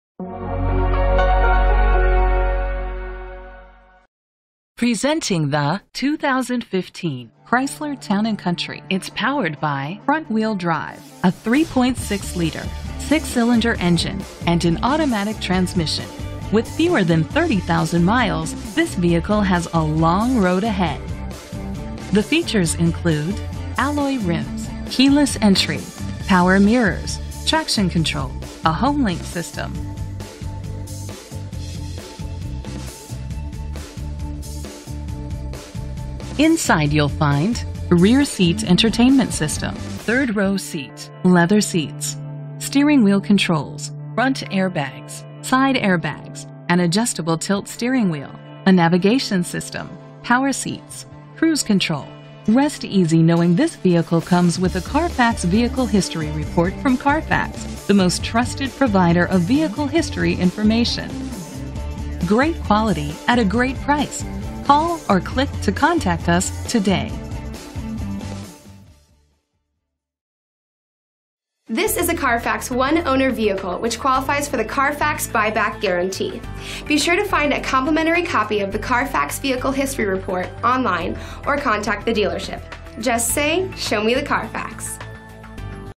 2015 Chrysler Town & Country F5044 - Tinley Park IL
Year: 2015
Make: Chrysler
Model: Town & Country
Trim: Touring
Engine: 3.6 liter 6 cylinder FWD
Transmission: Automatic
Color: Gray
Mileage: 28213
Address:
17514 Oak Park Avenue
Tinley Park, IL 60477
VIN:2C4RC1BG6FR644650